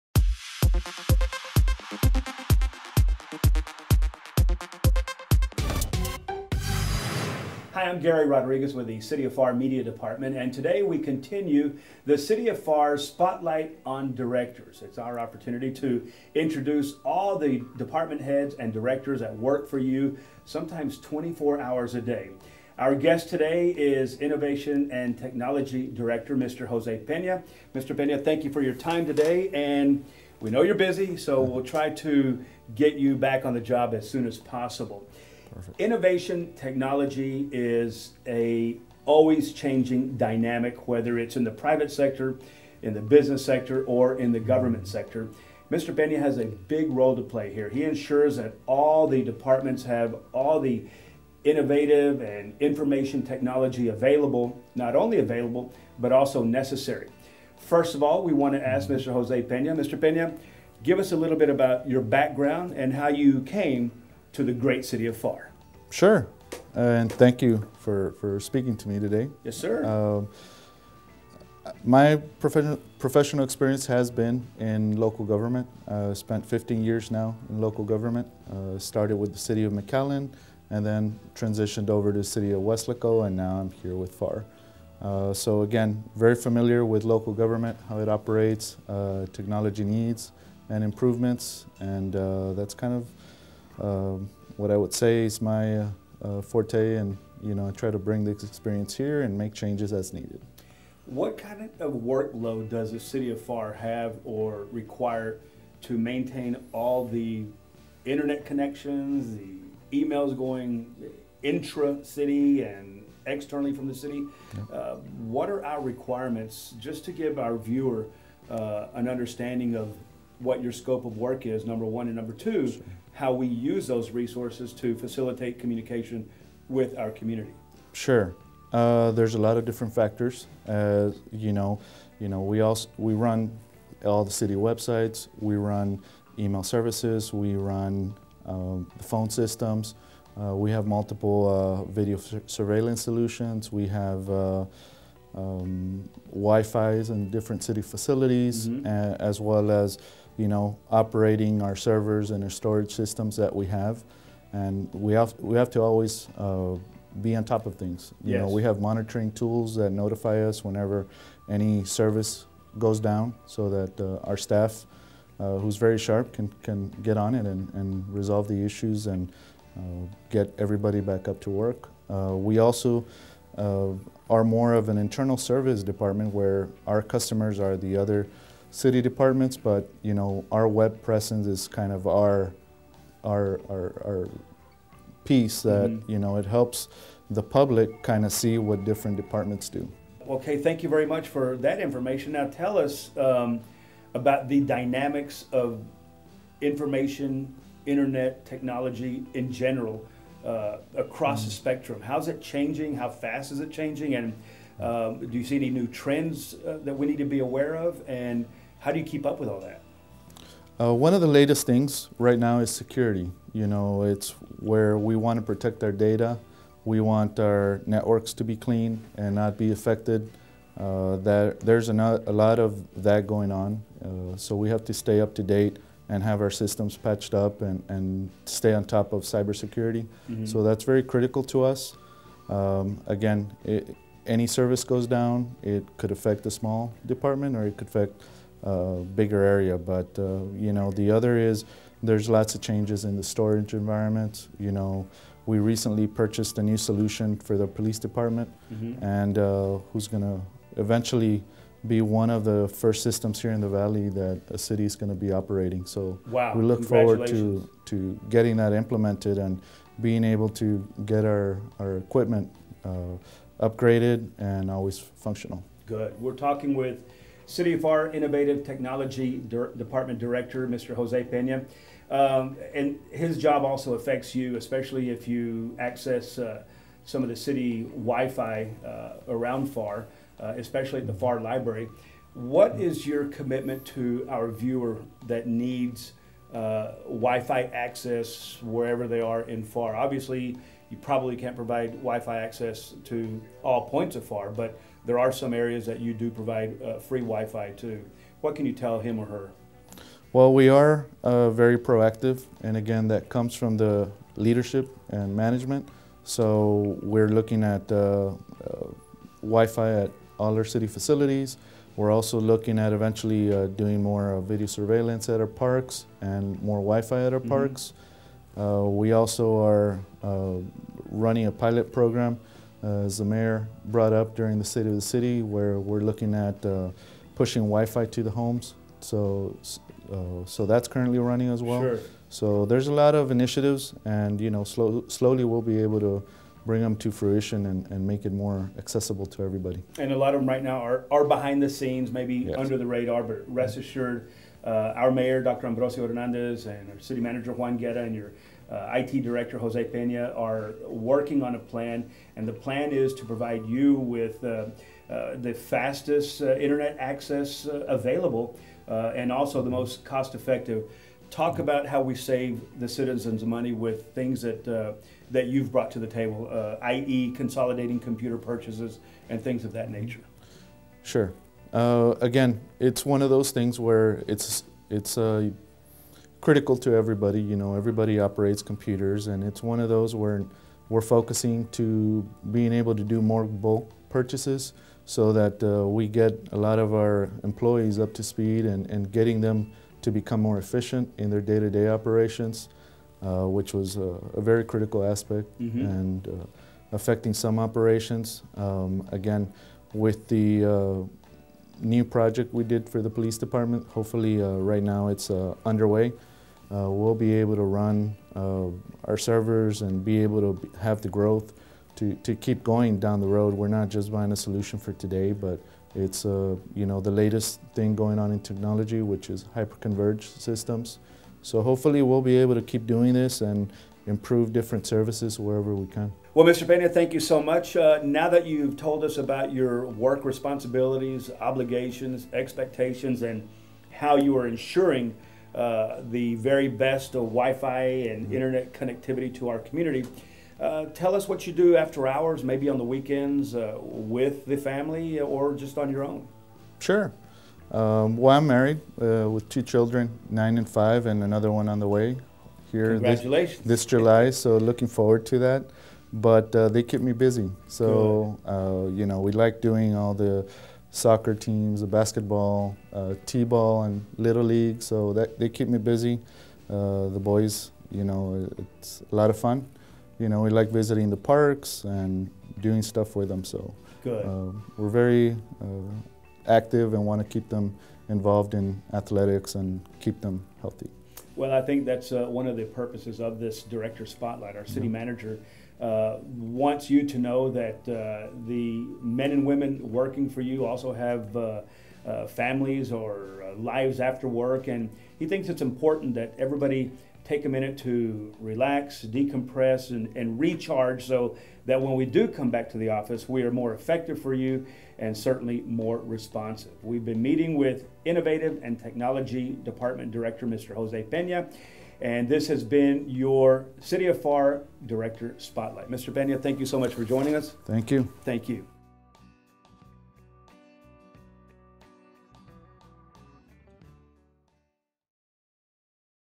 Department Head Spotlight - Jose Pena | City of Pharr
In this installment of Department Head Spotlight, we meet Jose Pena, the Director of the City of Pharr's Information & Technology or IT Department. Gary Rodriguez interview Jose and asks probing questions about who he is and what his department does to ensure that the City of Pharr's network and cyber infrastructure is intact.